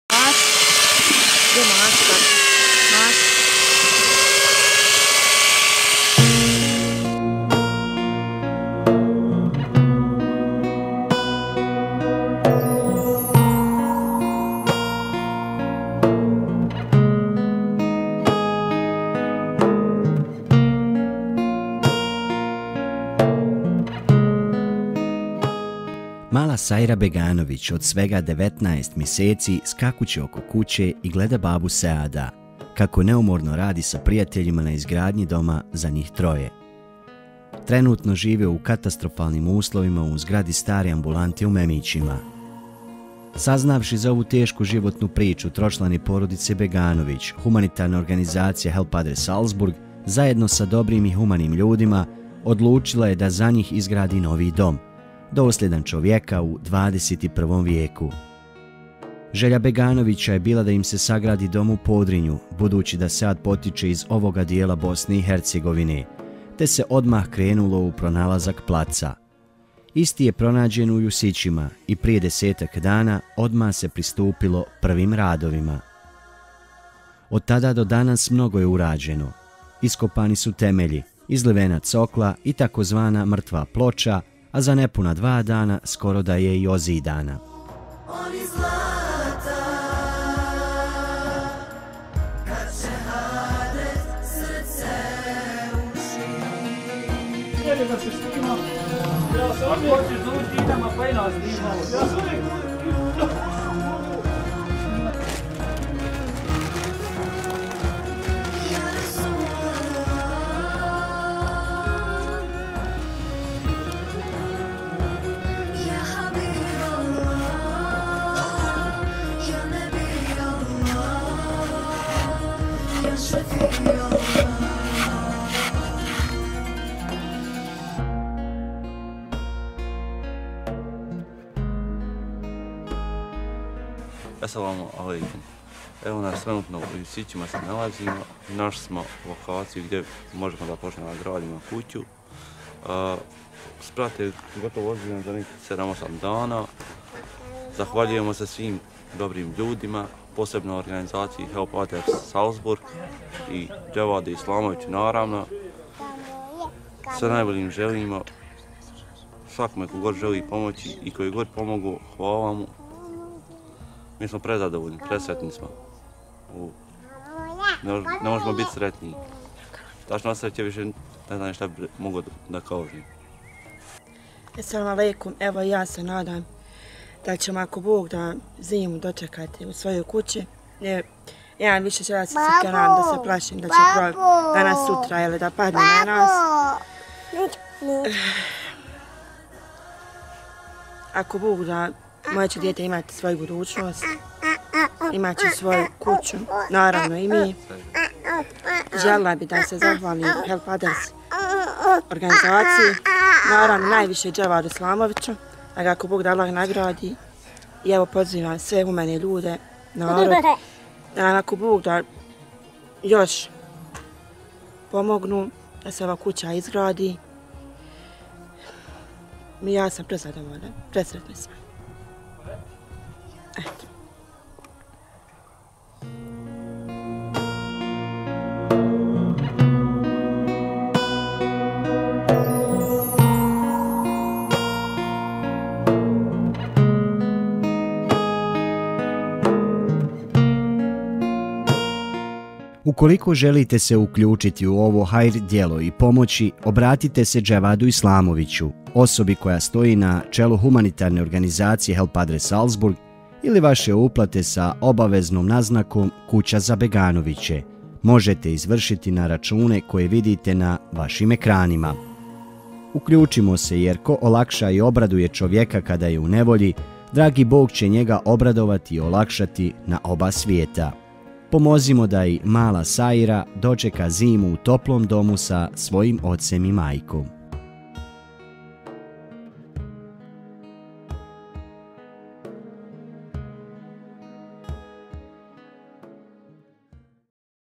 Hitan apel "Kuća za Beganoviće" -zidanje kuće
U toku je akcija izgradnje kuće za tročlanu porodicu Beganović, oca Seada, majku Lejlu i 19-to mjesečnu kćerkicu Sajru.

Nakon pribavljanja placa u naselju Jusići kod Zvornika za desetak dana urađeno je mnogo posla, ali ima tu još mnogo, mnogo toga da se radi.

Pomozite. Uključite se